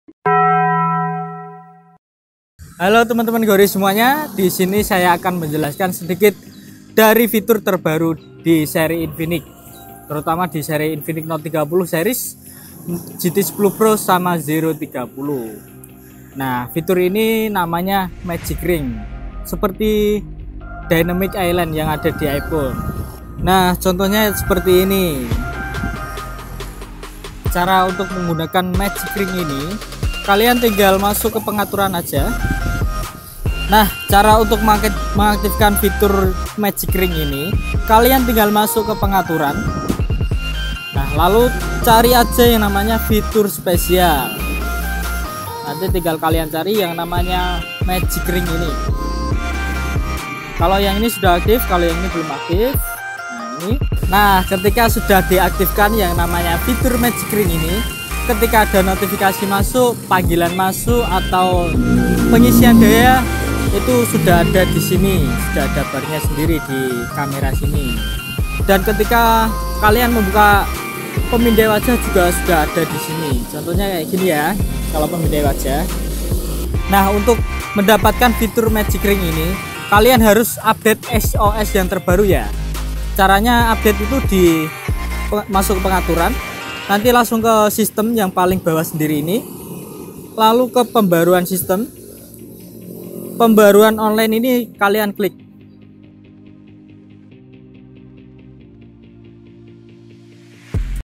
FITUR MAGIC RING INFINIX
Fitur terbaru dari Infinix Note 30 Series, GT 10 PRO, ZERO 30 yaitu fitur Magic Ring
Untuk mengaktifkan fiturnya simak video ini ya guyss

📍GORICELL

Beli HaPe di GORICELL bisa dapat sepeda listrik 🥳🥳